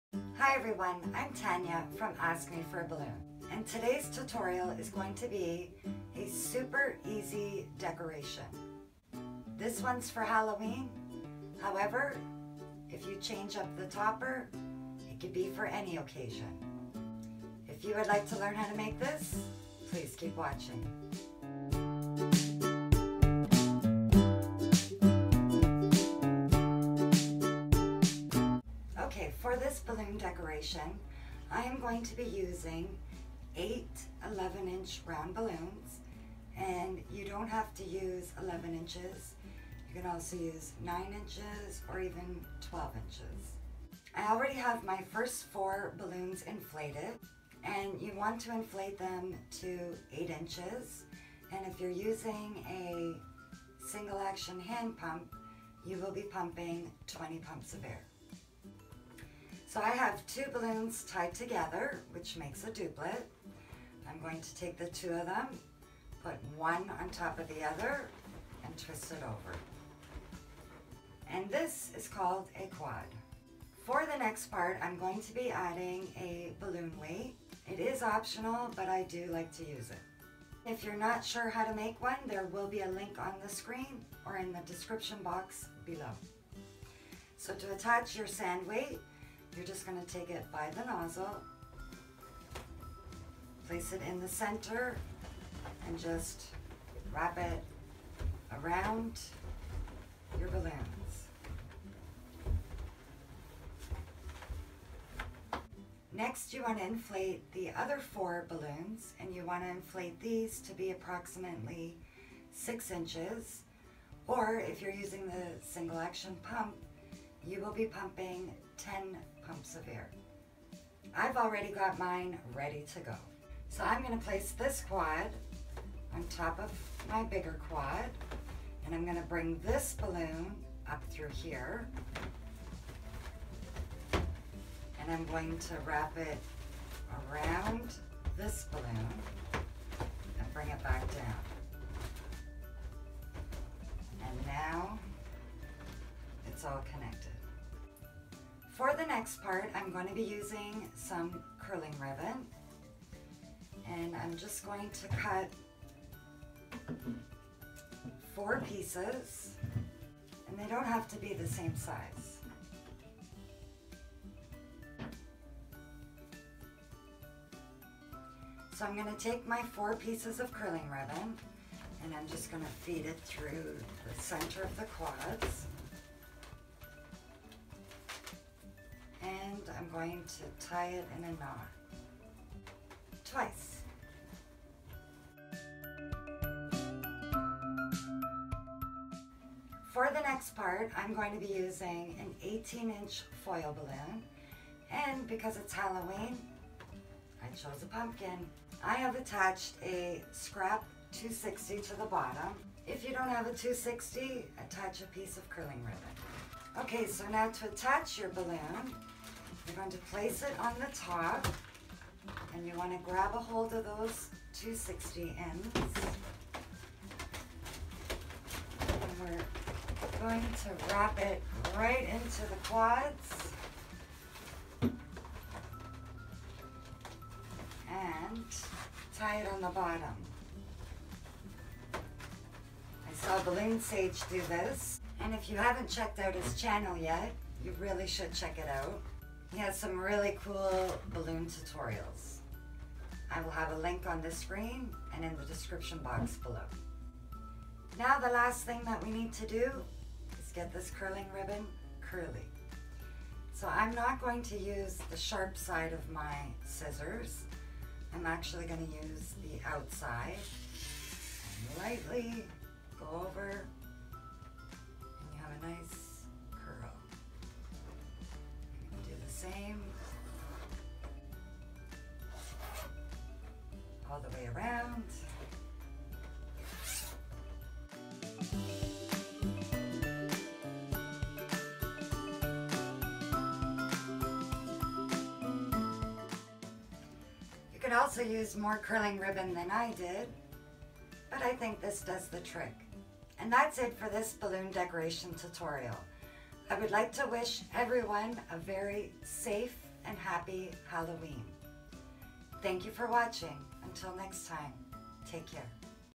Easy Balloon Decoration Tutorial - Party Idea
Learn how to make an easy balloon decoration with this tutorial. This one is for Halloween since I used a pumpkin foil, however if you change the foil topper it can be for any occasion! Make this for kids birthday parties, baby showers and more!